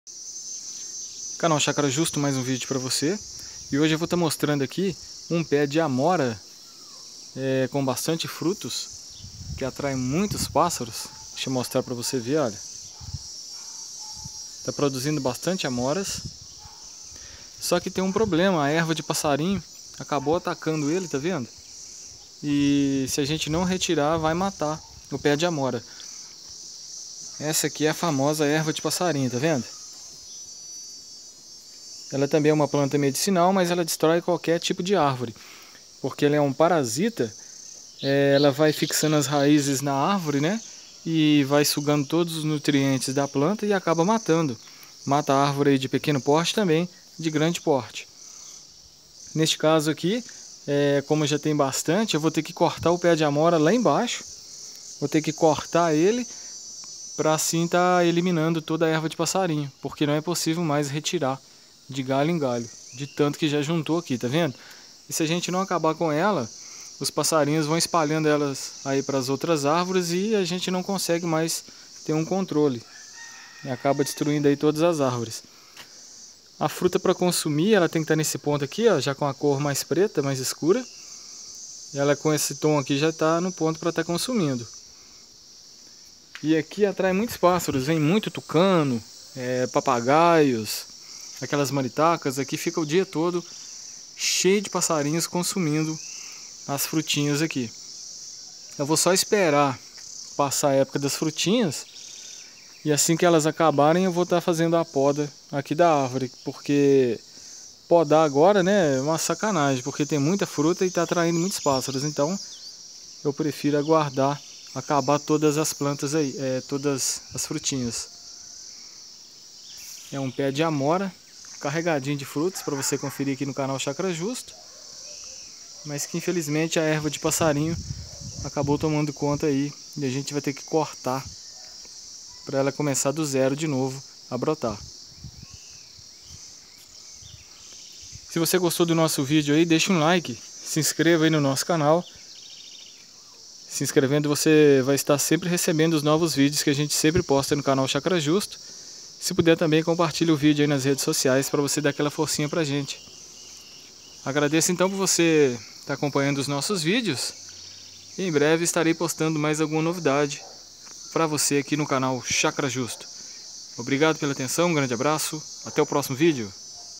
Pé de amora carregado de frutas destruído pela planta erva de passarinho.Linda árvore frutífera
. CONFIRA O NOSSO TIKTOK